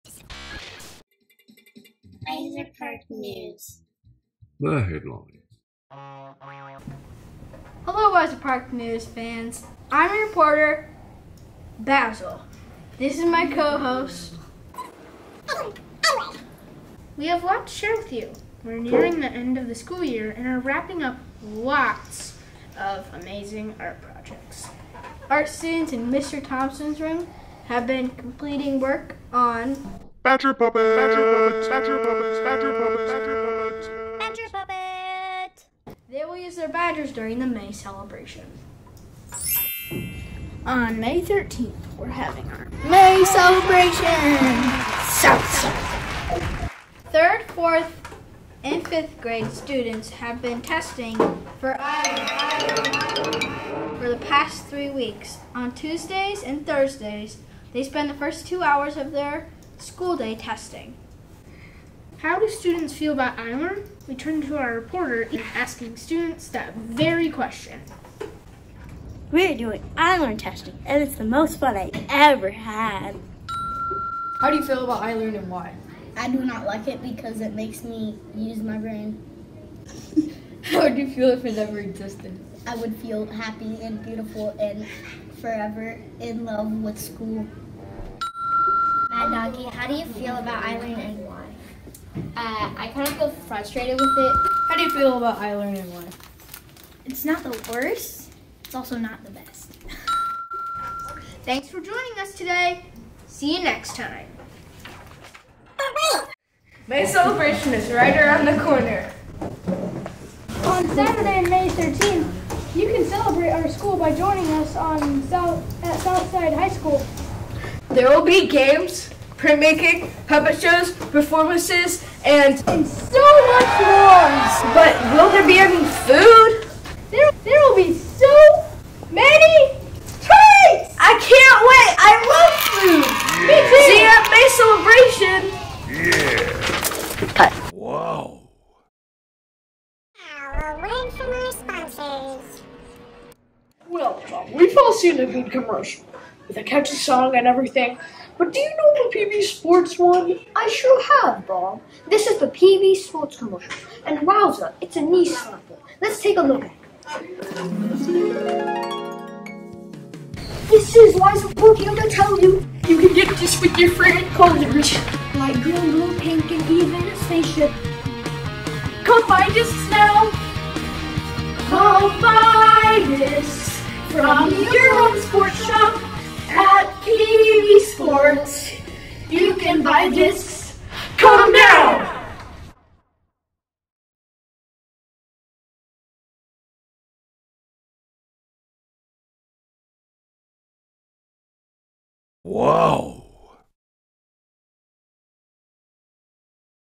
Weisser Park News (+Show!) S02E06
Media Arts students at Weisser Park Arts Magnet School in Fort Wayne, Indiana, report on school news and events. This episode's topics include May Celebration, an ILEARN survey, our PB Sports sponsored-disc golf course and end-of-the-year events!